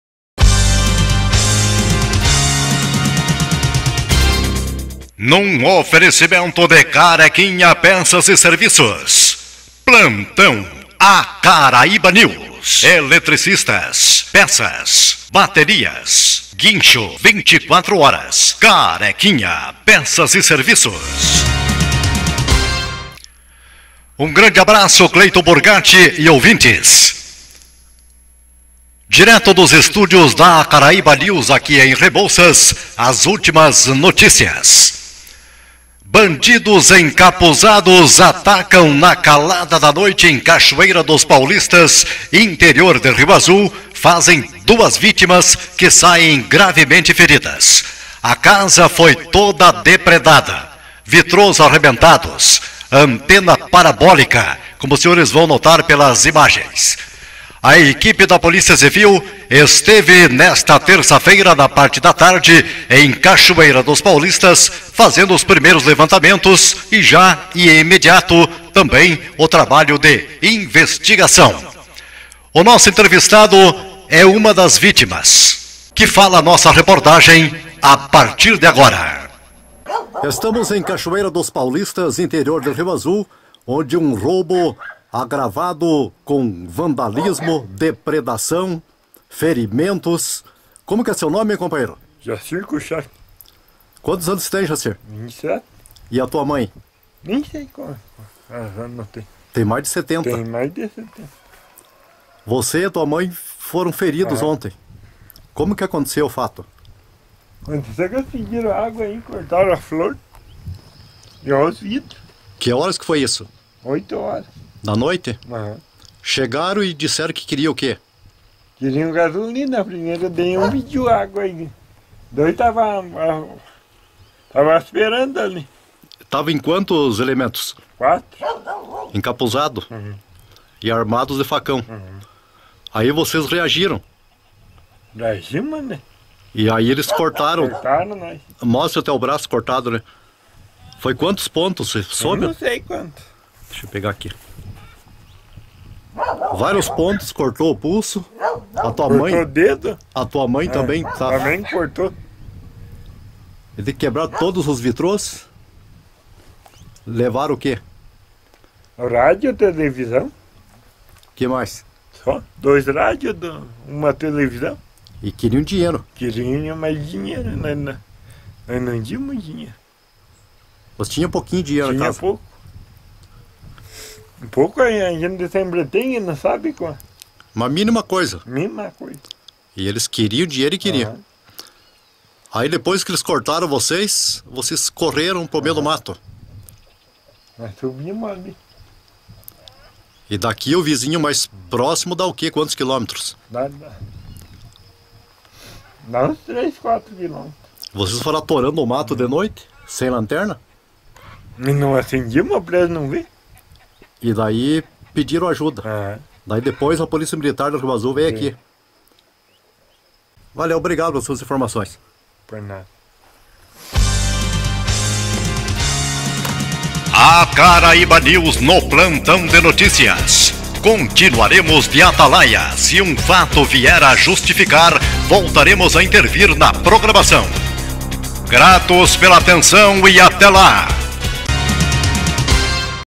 Assalto em Cachoeira dos Paulistas RIO AZUL Pr
Roubo, dois feridos no interior de Rio Azul Paraná.